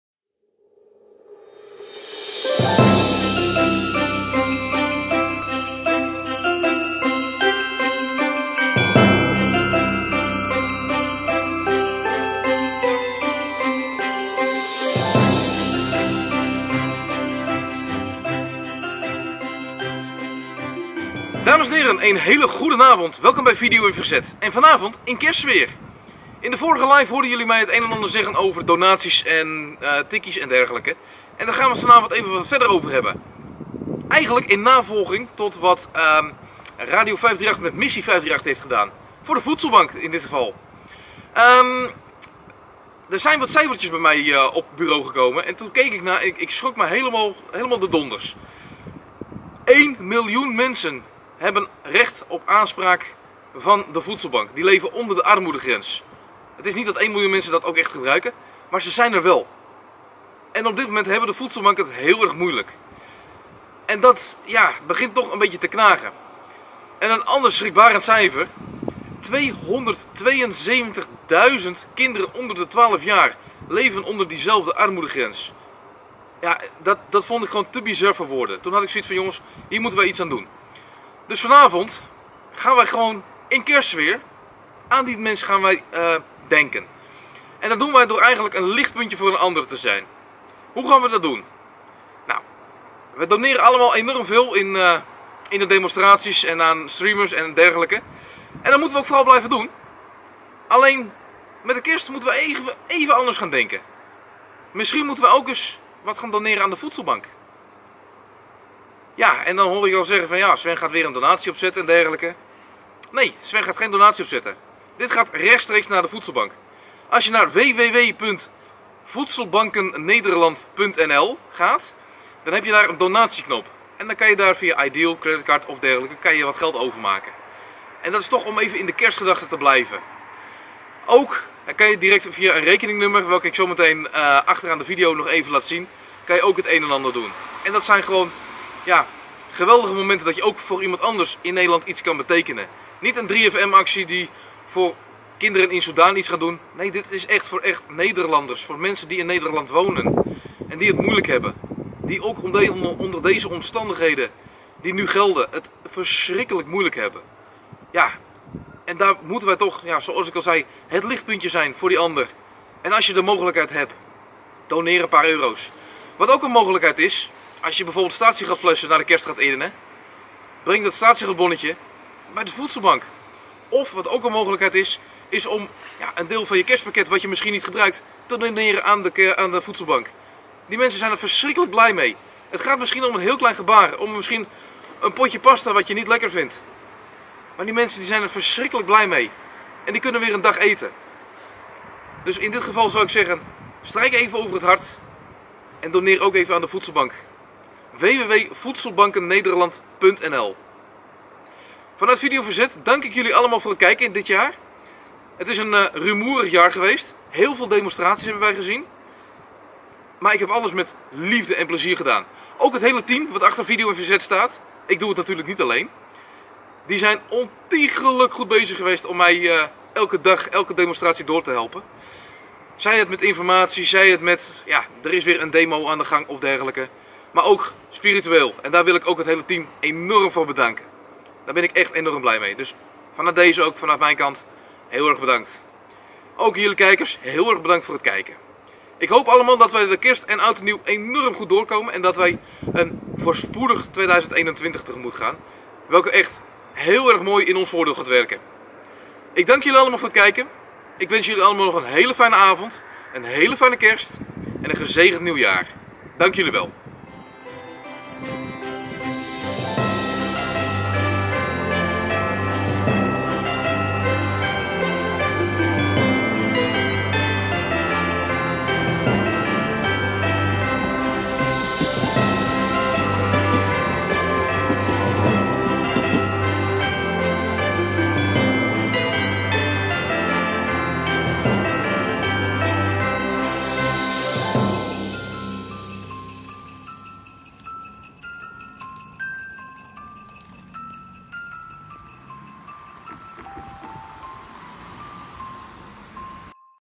Tikkie naar de voedselbank
Wat een jaar !!! ... maar nu genoeg over ons !
in navolging van Missie 538 roepen wij jullie op om waar mogelijk
iets te doneren aan de voedselbank
ga naar om te doneren of naar je lokale voedselbank.
Denk ook aan de restanten van je kerstpakket of je statiegeld bonnetje... kleine dingen kunnen al een groot verschil maken !!!

Vermeld vooral in de comments wat je hebt gedoneerd !!

Verder wenst Video In Verzet iedereen een hele fijne kerst en een voorspoedig 2021 !!